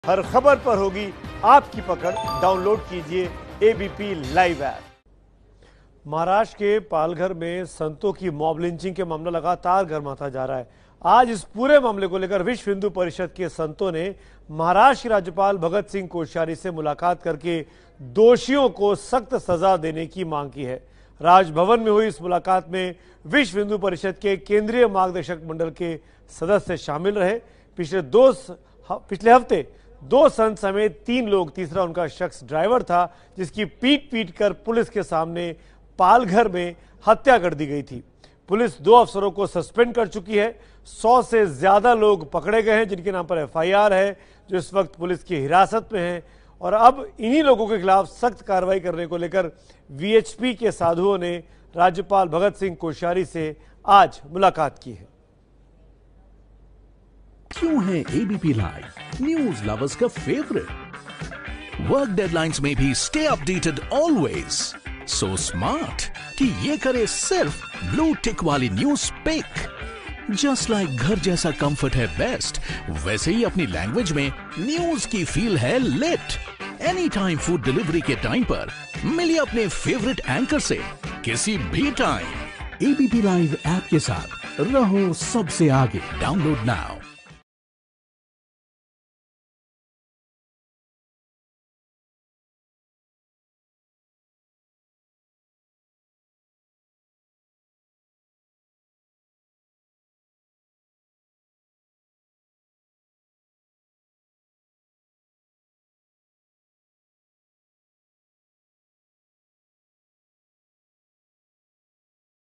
Palghar लिंचिंग को लेकर VHP के संतो ने Maharashtra के गवर्नर से मुलाकात की | ABP News Hindi
पालघर में हुई दो संतों और उनके एक ड्राइवर की मोब लिंचिंग के विषय में विश्व हिन्दू परिषद से जुड़े संतों ने महाराष्ट्र के राज्यपाल भगत सिंह कोश्यारी से मुलाकात की. संतों ने दोषियों को सख्त सजा देने की मांग की